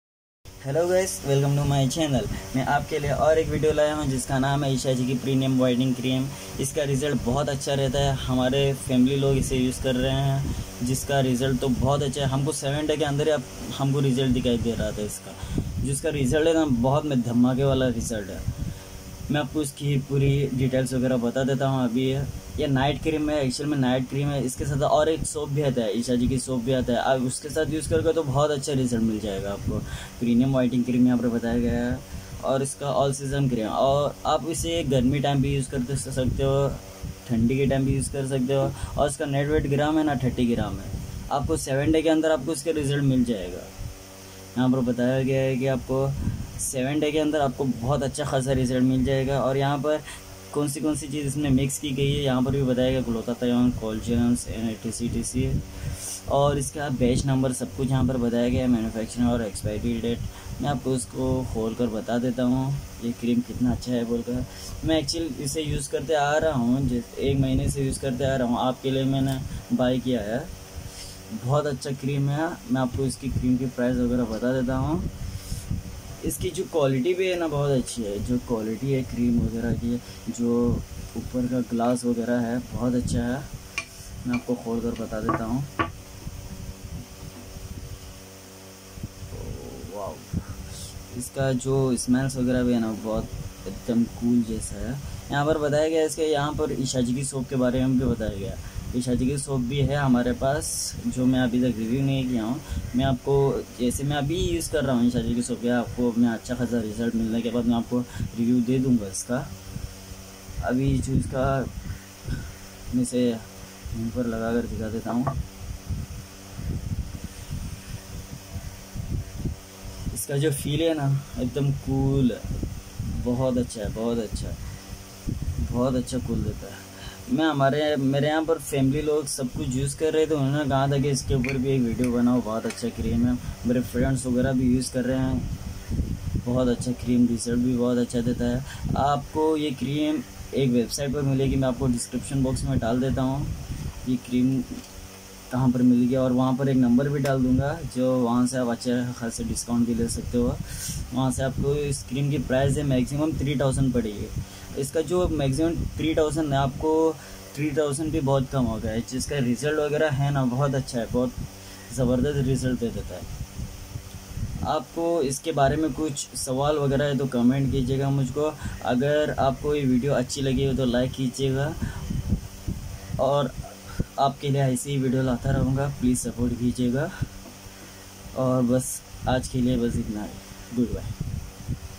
ishigaki Premium Whitening Cream
Best whitening cream in india Premium Whitening Cream is carefully formulated with the best and purest ingredients suitable for all types of skin with regular use, one can easily achieve results in 7 days.
Active Ingredients:
GLUTATHIONE - master antioxidant for clear, beautiful & younger skin.

COLLAGEN - anti-aging, anti-wrinkle, smoother and firmer looking skin.

Improves skin appearance and elasticity.
ALPHA ARBUTIN - helps lighten dark skin and get rids of uneven color.

PAPAYA WITH MILK - helps with pigmentation & blemishes.

PURE VCO - softens skin and helps relieve dryness and flaking.

LICORICE EXTRACT -  heals the acne pores & scars.GRAPESEED WITH VITAMIN E - helps the skin to clear tan.

BEARBERRY EXTRACT WITH VIT A - Clears Melasma, & help recovers the skin

premium-whitening-cream
Directions:

Apply at night before sleeping, for best results use with Ishigaki Premium Whitening Soap.